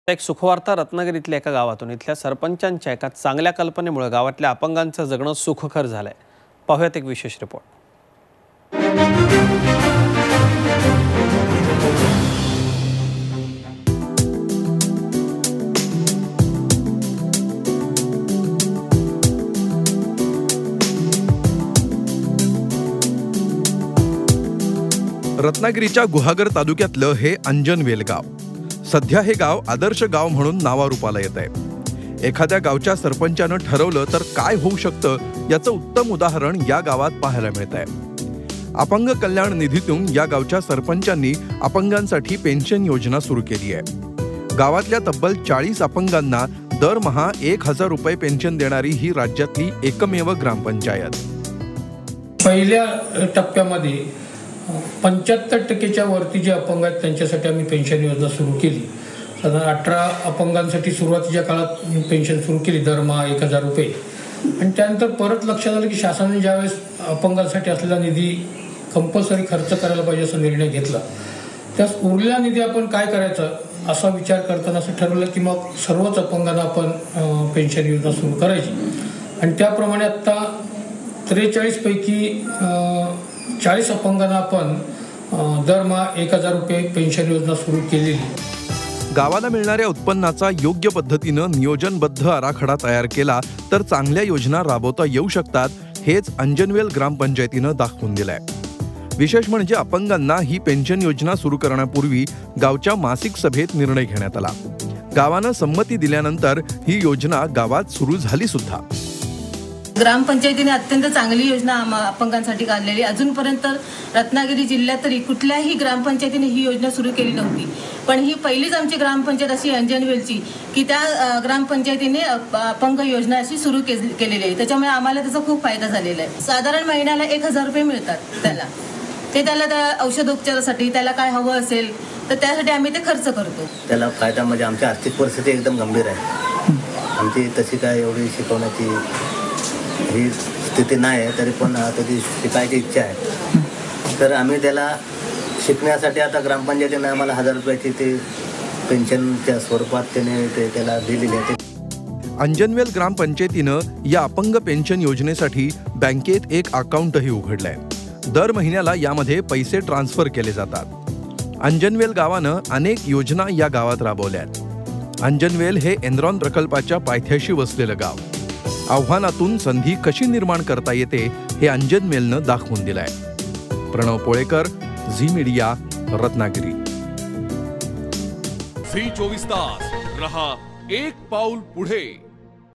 एन्रॉनच्या पायथ्याशी आव्हानातून संधी शोधणारं 'अंजनवेल'
Ratnagiri | Anjanvel Grampanchayat Giving Rupees One Thousand As Pension To Handicappe Peopl